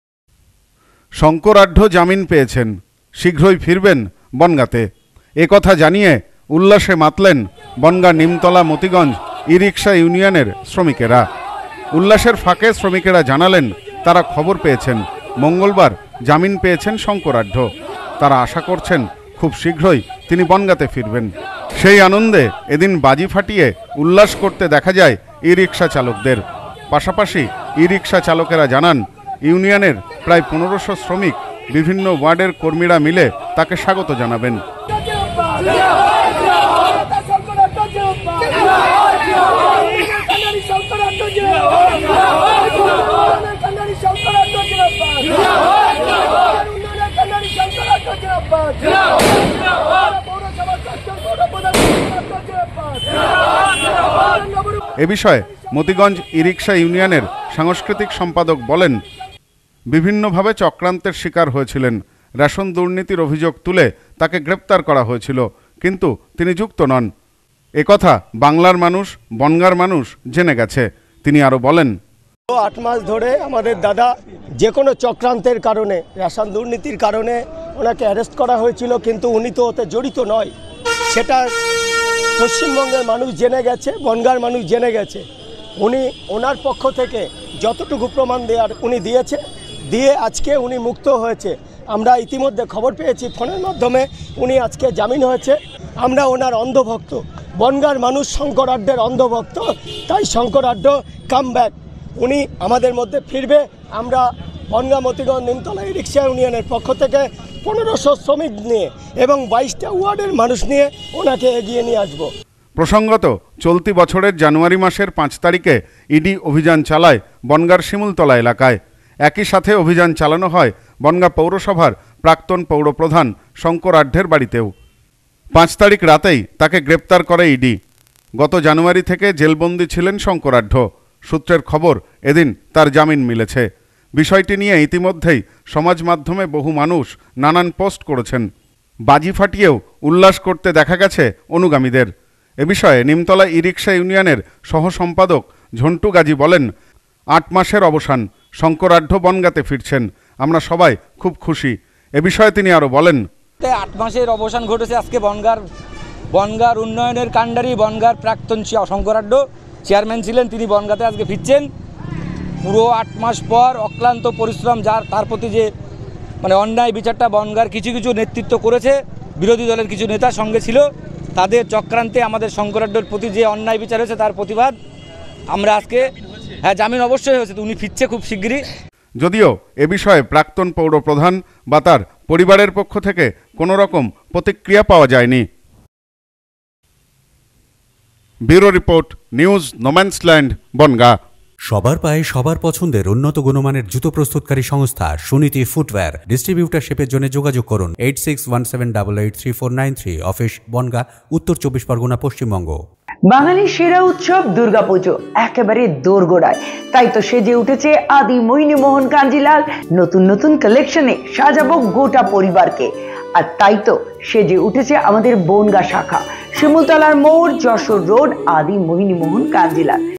শংকর আঢ্য জামিন পেয়েছেন, শীঘ্রই ফিরবেন বনগাঁতে।
শংকর আঢ্য জামিন পেয়েছেন, শীঘ্রই ফিরবেন বনগাঁতে। একথা জানিয়ে খুশিতে মাতলেন মতিগঞ্জ ই-রিক্সা ইউনিয়নের সদস্যরা। ফুটছে বাজি, সমাজ মাধ্যমে শুভেচ্ছা বার্তা।